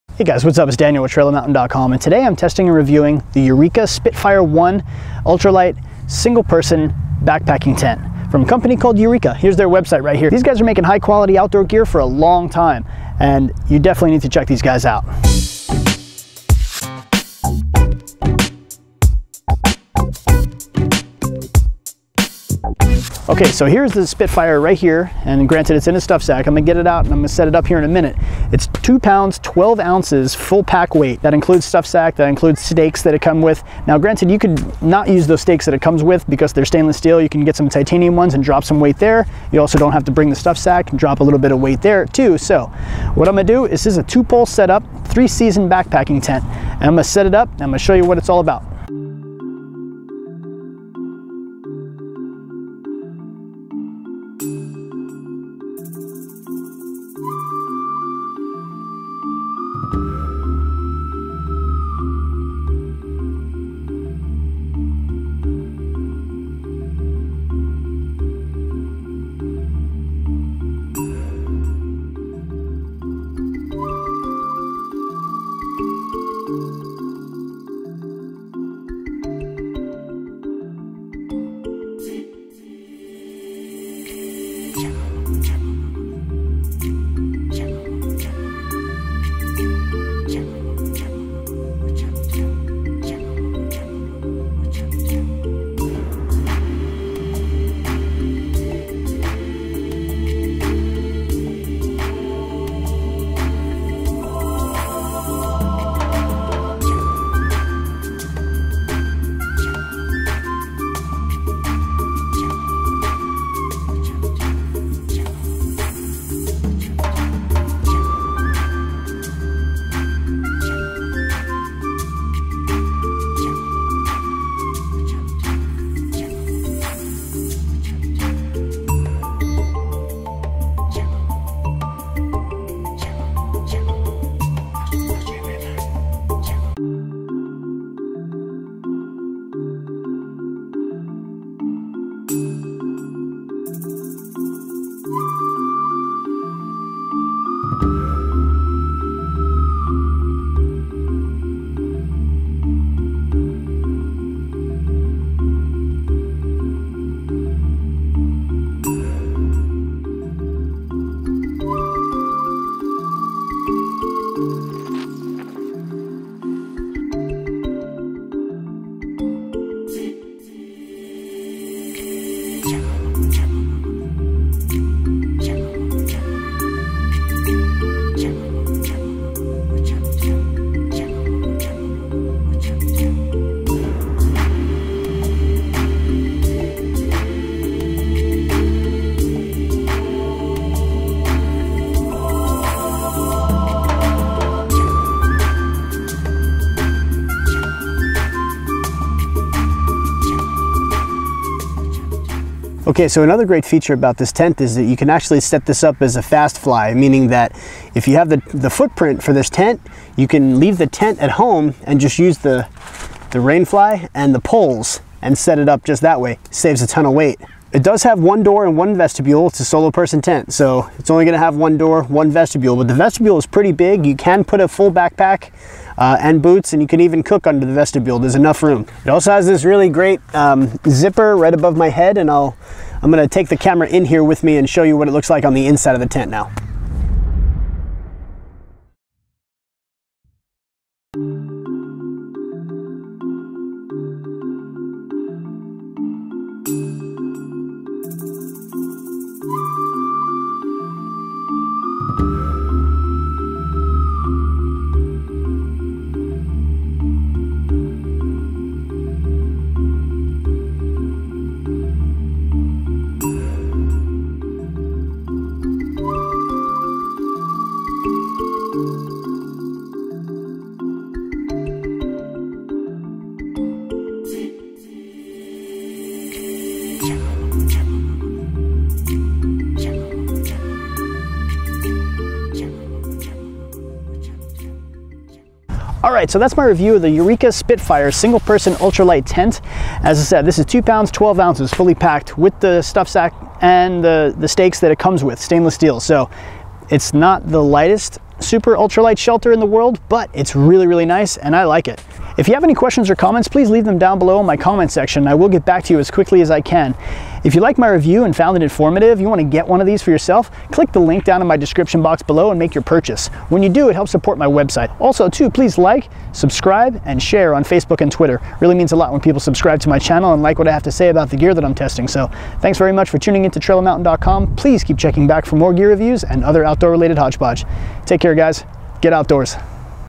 Eureka Spitfire 1 Solo Tent Gear Review
This is by far one of the best constructed solo tents I have ever reviewed. The simplicity and ease of use built into this tent is awesome. Its lightweight and very well made. The only thing I would add would be a gear loft as there isn't that much room...other than that its a great solo tent thats inexpensive and works great!